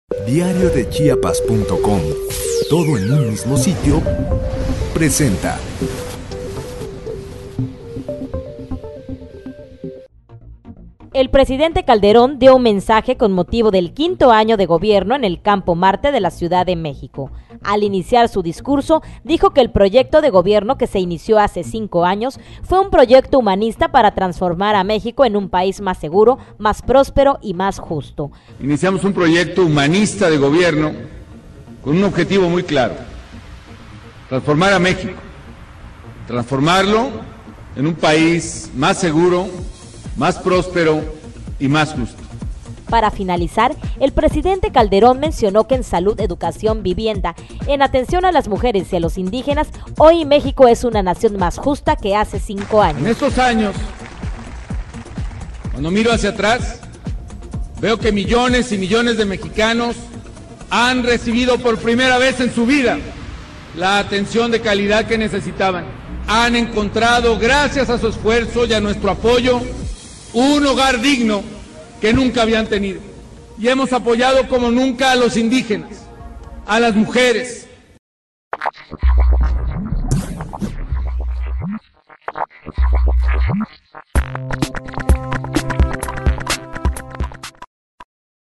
5° año de Gobierno Felipe Calderon Hinojosa
El presidente Calderón dio un mensaje con motivo del Quinto Año de Gobierno en el Campo

En el campo Marte ante diputados, senadores y gobernadores entre ellos Juan Sabines Guerrero.

Marte de la Ciudad de México.
Al iniciar su discurso, dijo que el proyecto de gobierno que se inició hace 5 años fue un proyecto humanista para transformar a México en un país

más seguro, más próspero y más justo. Reconoció que uno de los mayores desafíos a los que se ha enfrentado es el de la inseguridad.